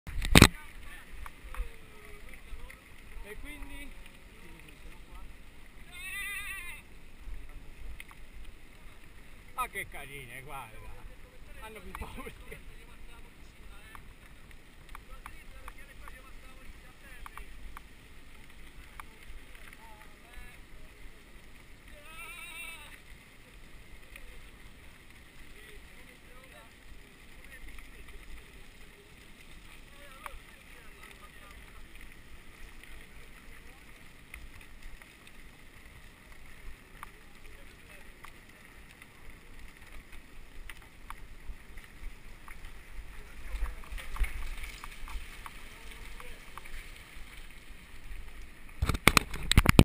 Maccarese bike day Settembre 2016 (XI)
Bellissima passeggiata insieme ai ragazzi di FIAB Roma Naturabici, Pedalando, Salva i ciclisti,.

Con le capre pure!